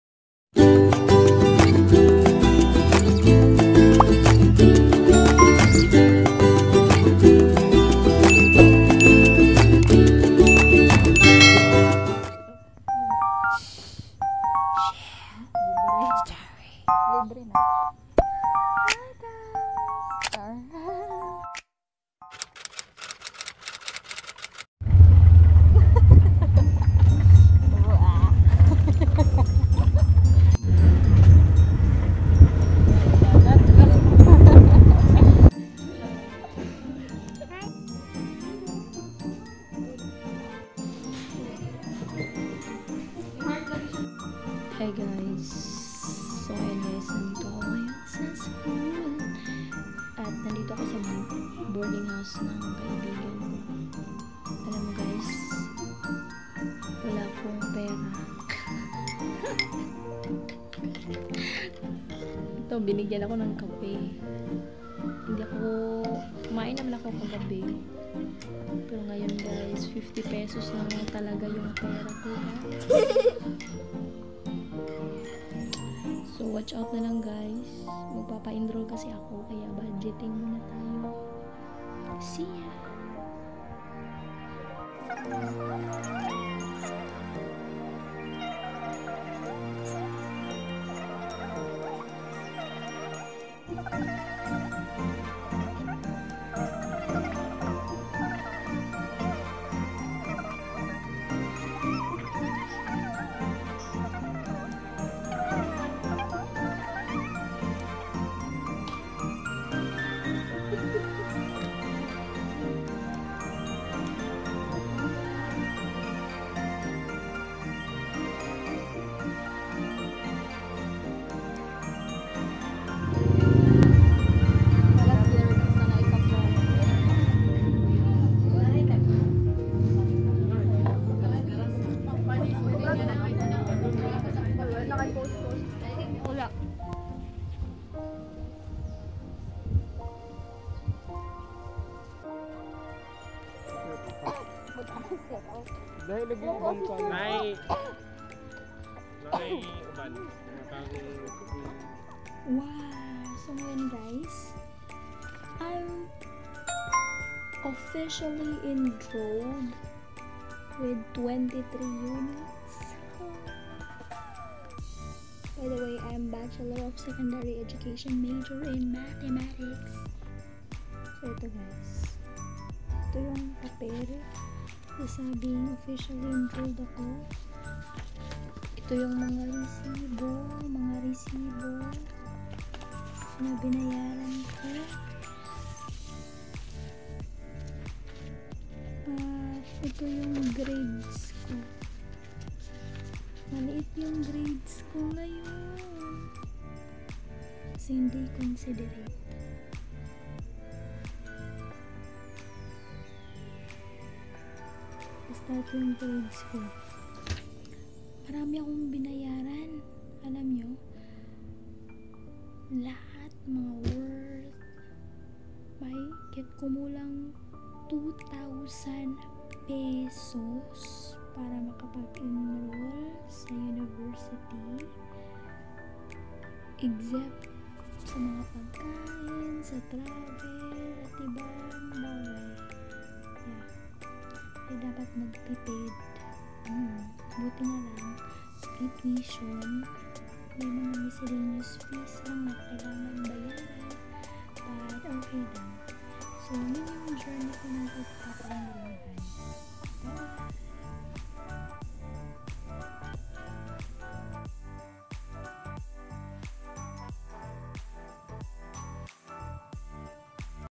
Enrollment Day sa aming Unibersidad 🙂
Strict Covid-19 protocols are emphasized in the area. Fully vaccinated individuals can only enter. Face mask and Face shield are strictly mandated to wear.